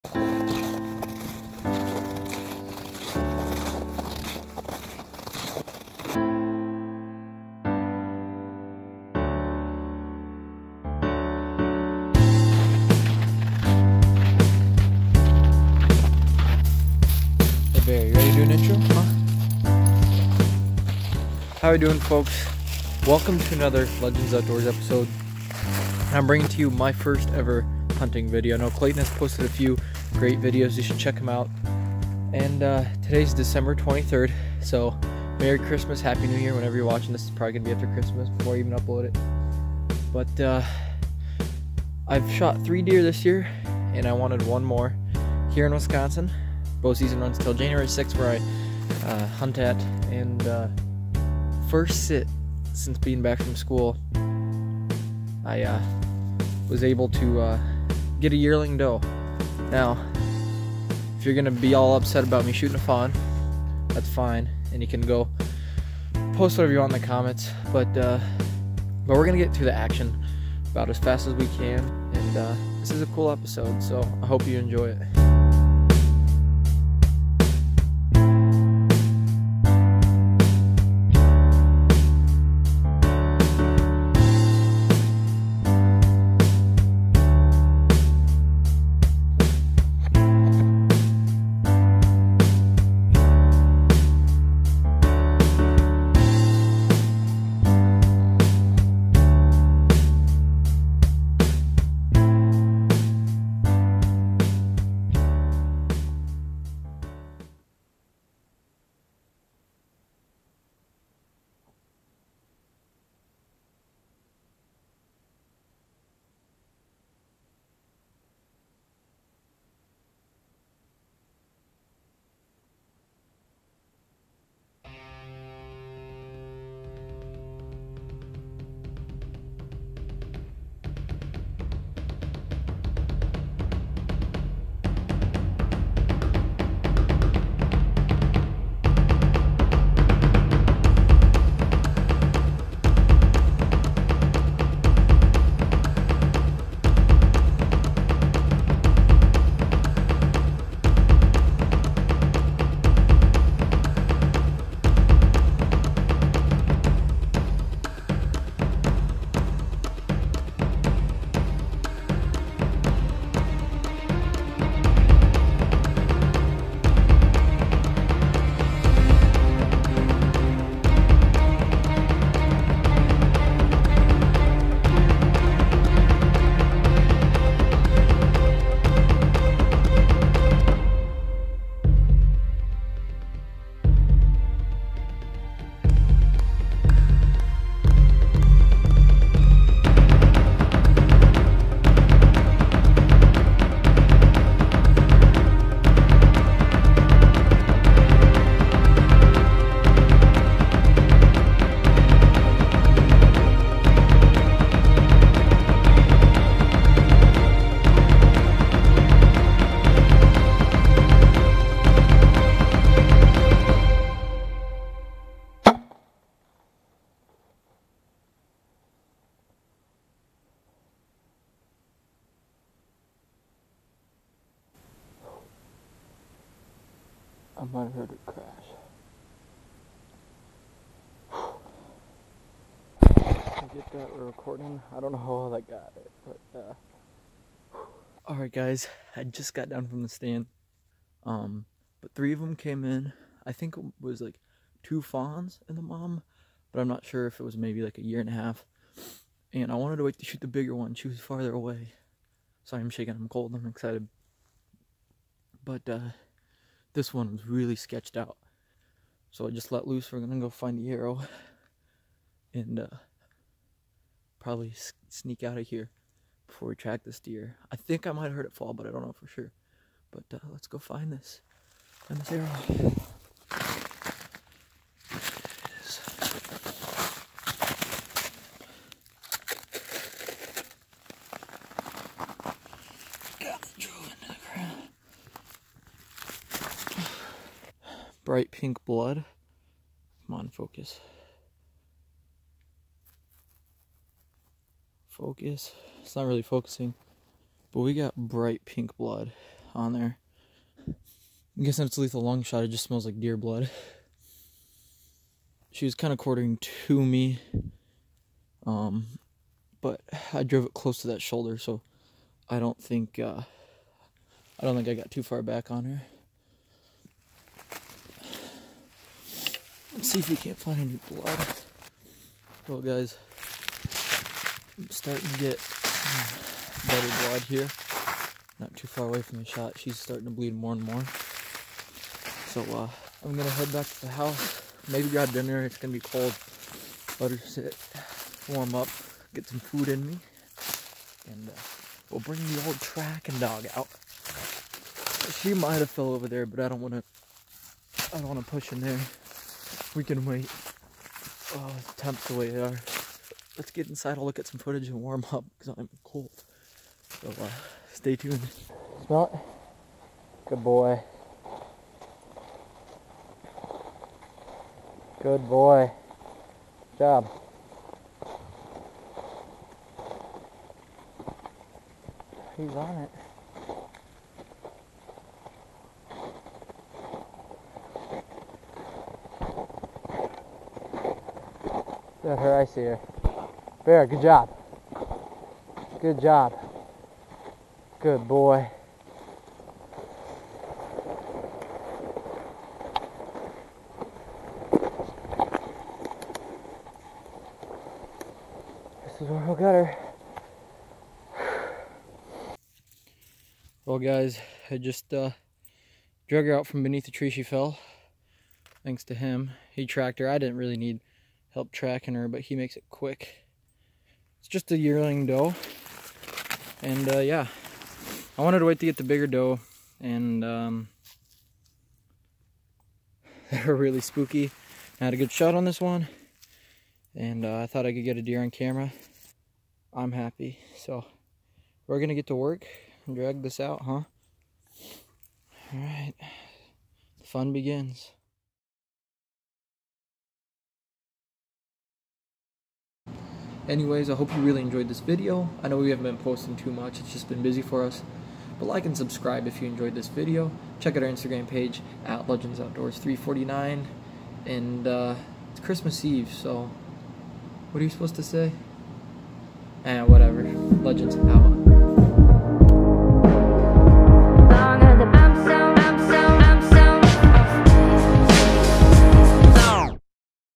Late Season Bow Hunt | 2018 WI Bow Season
Legends Outdoors member, Nathan, headed out to his stand recently to fill another anterless tag.  With Wisconsin bow season winding down, he was lucky to shoot a deer on his first sit.

WARNING: This video contains a fawn being shot.